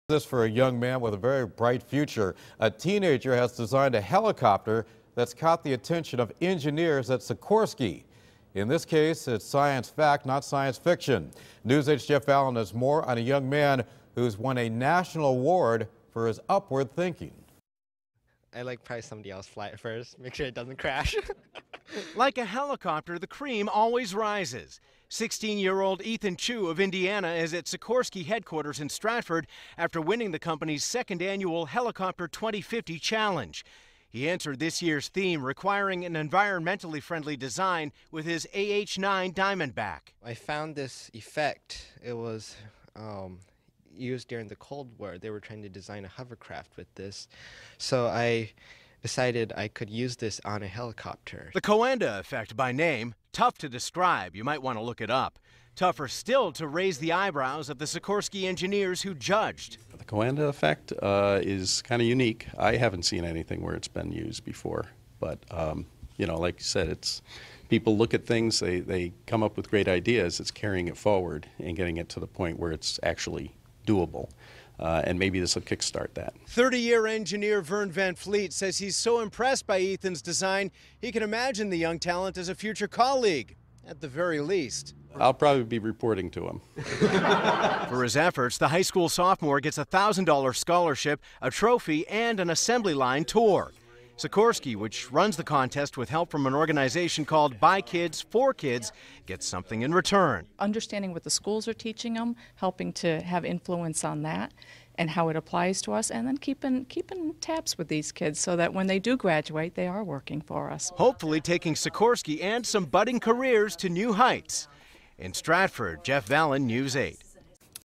Teen wins Sikorsky helicopter challenge
Imagine a teenager designing a helicopter that catches the attention of Sikorsky engineers. Well, it's science fact, not fiction.